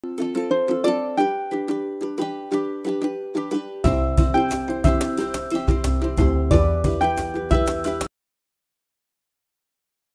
Add Style to Your Walls: Wooden Shelf Brackets for Home
Enhance the beauty of your home decor with our wooden wall hanging shelf brackets. Made from premium wood, these brackets are both durable and stylish. Whether you want to display photos, plants, or other decorative items, these brackets will add a touch of elegance to any room.